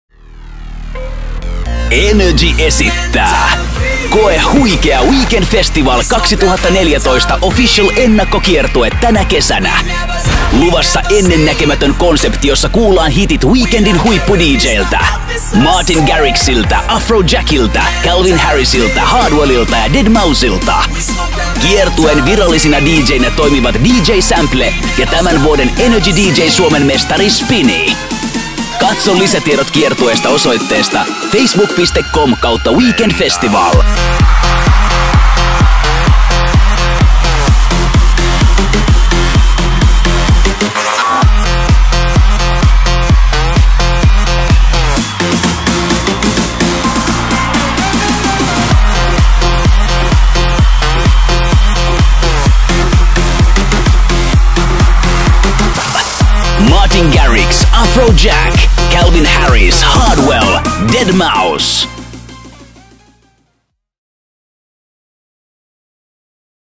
Weekend Festival 2014 Pre Tour
Tänä vuonna Weekend Festival kiertää ympäri Suomea uudenlaisella konseptilla jossa kuullaan Weekend Festivalin huippuartistien setit Suomen parhaisiin klubi DJ:hin lukeutuvien DJ SAMPLn ja tänä vuonna toistamiseen DJSM -kilpailun voittaneen DJ Spinnyn tulkitsemina.

Bileissä kuullaan mm. Afrojack, Martin Garrix, deadmau5, Calvin Harris ja Hardwell setit ja bilekansa pääsee virittäytymään Weekend Festivalin tunnelmaan jo etukäteen näyttävän tuotannon siivittelemänä!

WKND 2014 - OFFICIAL PRE TOUR KIERTUEPÄIVÄMÄÄRÄT JA KAUPUNGIT:

PE 11.7. LAHTI TIVOLI w/ DJ SAMPL
LA 12.7 KAJAANI ONNELA W/ DJ SPINNY

PE 18.7 JYVÄSKYLÄ GIGGLING MARLIN w/ DJ SPINNY
LA 19.7 OULU TIVOLI w/ DJ SPINNY

PE 25.7 LAPPEENRANTA GIGGLING MARLIN w/ DJ SAMPL
LA 26.7 KUOPIO ONNELA w/ DJ SAMPL

BIO

DJ SAMPL
3 Times Finnish DJ Champion 2012, 2002, 2001.
Voted Top 10 Best DJ's in Finland 2012 (Klubikysely).
Voted #12 DJ's in Finland 2013 (Top 100 Finland).
DJ'ing since 1992.

DJ SPINNY
2 Times Finnish DJ Champion 2013, 2014.
2012 #3 & Scratch DJ of The Year
DJ'ing since 2008

LIPUT - OVELTA

Lisätiedot tapahtumapaikkojen yökerhojen Facebook / kotisivuilta.